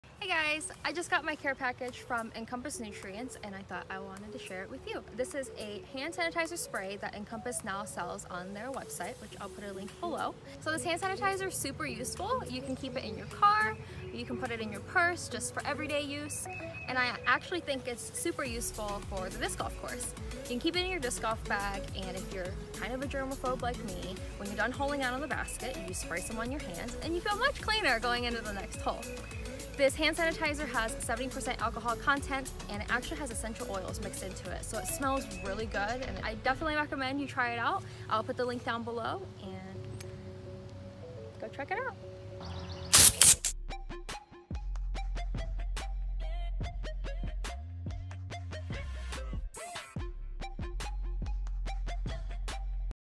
Currently, The Most Used Item In My Disc Golf Bag Is...
I love to play disc golf, but during these current times- things are a little sketch.  There are germs EVERYWHERE and I feel like we never considered that until now.

Playing on a public disc golf basket makes me a bit nervous, so I keep this Hand Sanitizer Spray on me during my round!

It's super easy to use and it SMELLS GREAT- due to the essential oil blend.
Importantly, the alcohol content is 70%!  Most leading brands only have about 62% which technically is the bare minimum to kill bacteria.

Click on the link and go try some out!
Might I also suggest that their Complete Adult + Joint Health make for a healthy decision as well!